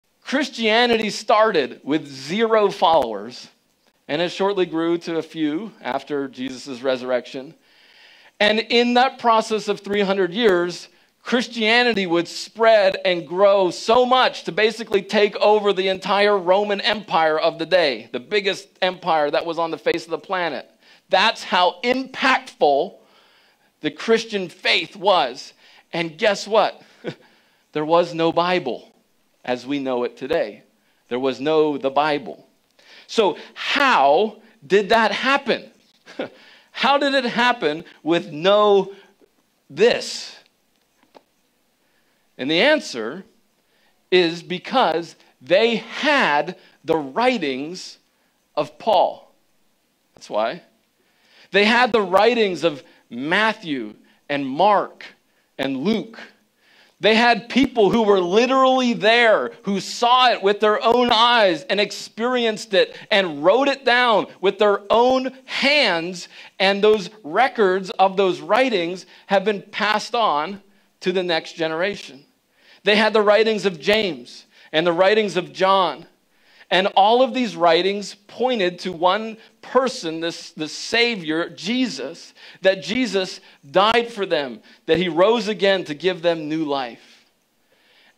Without "The Bible" As We Know It Today?
The Christian faith spread throughout the largest known empire on the face of the planet without "the Bible" (as we know it today). We have to understand what the Bible is and what it is not in order to build our life on it. What our Bibles contain today made this all possible.